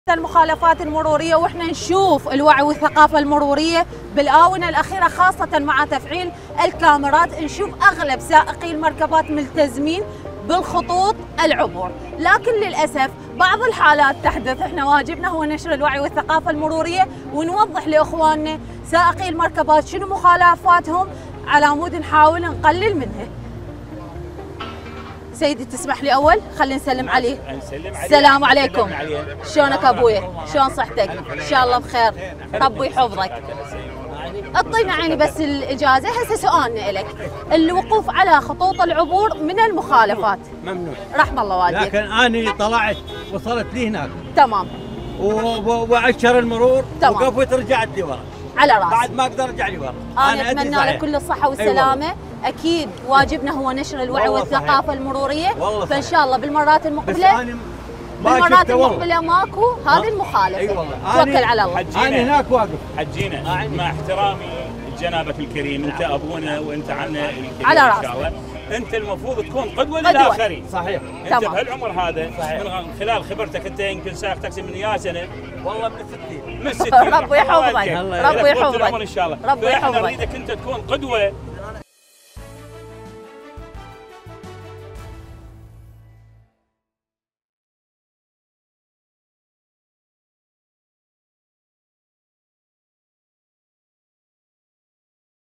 رسالة شرطة المرور لسائق تاكسي مسن وقف على خط العبور في بغداد
"انت أبونا"

موقع قناة التغيير الفضائية  الالكتروني